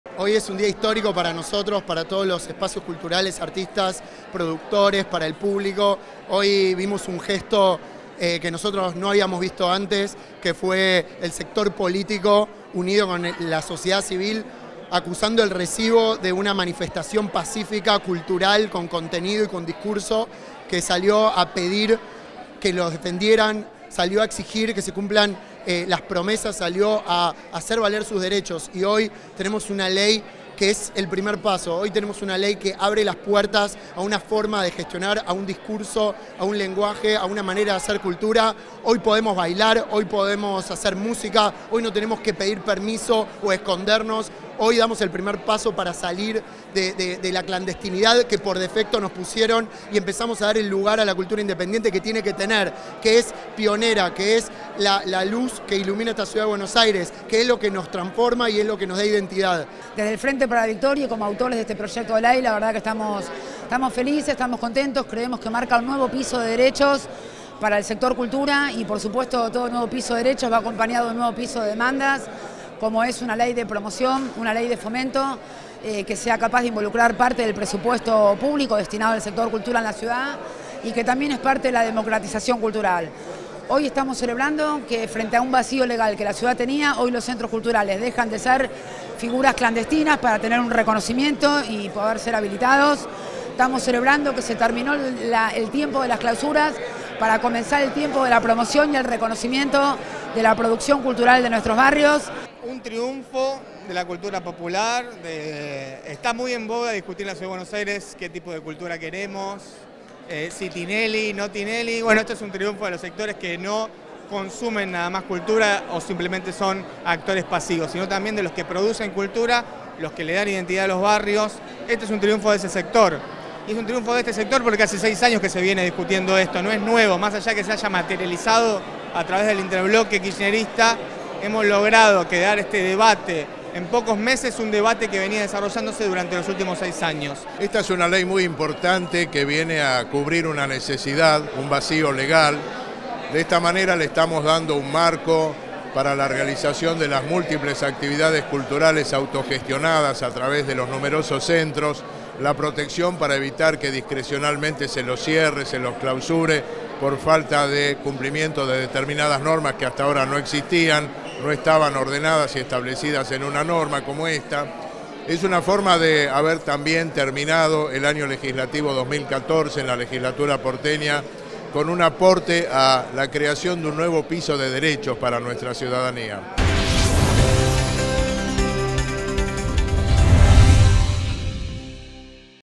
Tras las clausuras del gobierno porteño aprueban por unanimidad la ley de centros culturales
Los legisladores de la Ciudad de Buenos Aires convirtieron hoy en ley y por unanimidad el proyecto del kirchnerismo que otorga un marco regulatorio al funcionamiento de los centros culturales, tras las clausuras efectuadas por la Agencia Gubernamental de Control local de esos espacios por irregularidades con la habilitación.
La iniciativa, elaborada en conjunto con el colectivo de artistas y centros culturales y artísticos de toda la Ciudad, quedó aprobada con 56 votos positivos en el marco de una sesión extraordinaria de la Legislatura que tuvo como otro tema la jura de los representantes de los abogados ante el Consejo de la Magistratura.
En ese marco, también se sumó a último momento el expediente que contempla el marco regulatorio de los centros culturales existente en la Ciudad, el que era reclamado por organizaciones como Unidos y Organizados y MECA.
La diputada Lorena Pokoik, del Frente para la Victoria, afirmó que el proyecto sancionado “recoge la demanda de los centros culturales y pone fin al período de clausuras de estos espacios en los cuales se expresa, se produce y se disfruta la diversidad y pluralidad cultural que caracteriza a la Ciudad de Buenos Aires”.
En tanto, la presidenta del bloque kirchnerista, Gabriela Alegre, aseguró que “esta ley es un símbolo de lo que podemos llegar a hacer cuando nos escuchamos para llegar a un consenso”.
Por su parte, el diputado por Seamos Libres, Pablo Ferreyra, celebró la aprobación de la ley dado que con la norma “se va a sacar de la clandestinidad a los centros culturales que tanto aportan a la identidad de la Ciudad y lograr su legitimidad en algo concreto”.
El proyecto abarca la incorporación de la figura de los centros culturales dentro del Código de Habilitación y modifica el Régimen de Planeamiento Urbano local, reforma que requerirá de una segunda sanción en el recinto en el 2015 tras el paso de la iniciativa por una audiencia pública para dar lugar a la expresión de los ciudadanos.
Asimismo, incluye un listado de actividades permitidas en los centros culturales y un régimen tributario especial para aquellas entidades que funcionen sin fines de lucro.
Para el legislador de Nuevo Encuentro Edgardo Form, la norma “es un paso muy importante porque viene a cubrir un vacío legal y crea un marco normativo que garantiza la estabilidad de los centros”; en tanto que el legislador de la Coalición Cívica Maximiliano Ferraro aseguró que la aprobación “expresa la lucha del movimiento culturales en la Ciudad y el no renunciar a los matices a la diversidad”.
Por último, Claudio Coreman, de MECA -uno de los espacios culturales que participó de la redacción de la ley- consideró la sanción como “un gesto del sector político que hizo acuse de recibo de nuestras manifestaciones que exigíamos salir de la clandestinidad y nos da un lugar a la cultura independiente”.